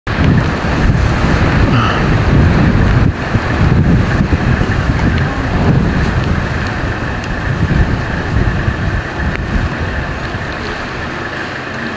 Leopard eating in tree
Leopard eating a kill she had stashed in a tree.